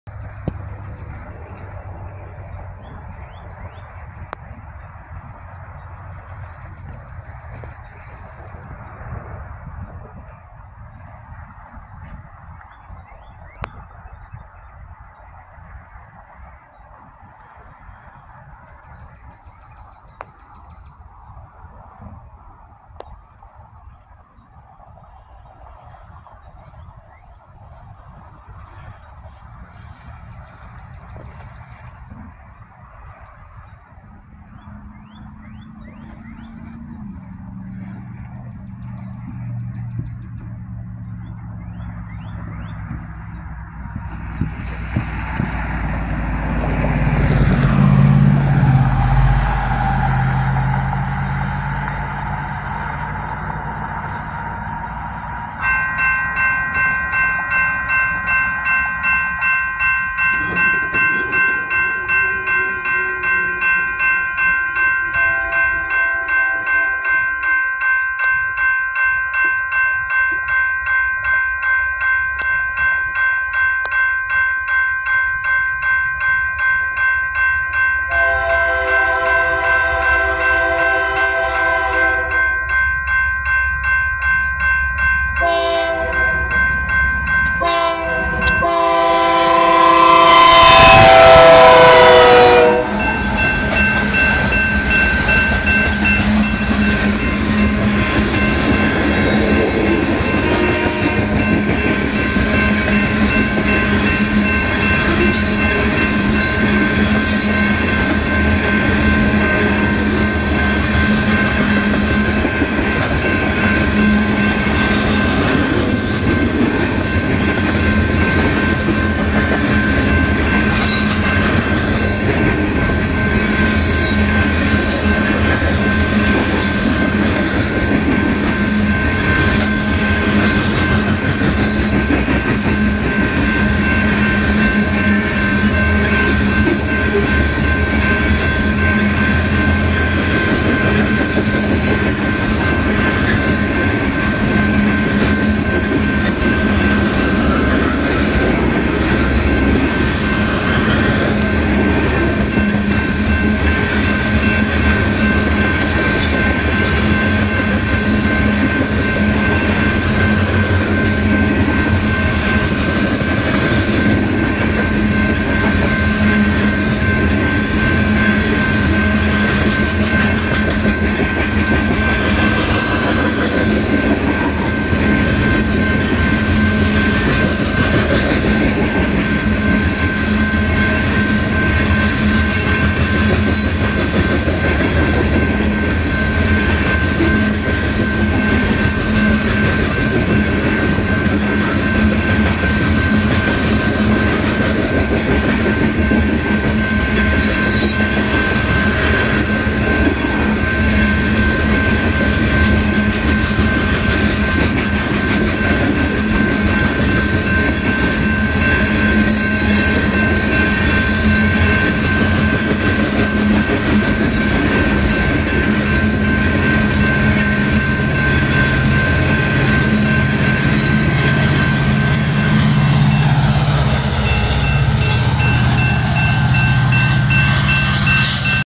UP #6201 Coal Train EB in Waterloo, NE (4-13-24)
Train information:
UP 6201 (Ex SP 153) Rebuilt GE C44ACM - Flags - Raised Letter K3HA
UP 8949 = EMD SD70AH - Flags (Rear DPU)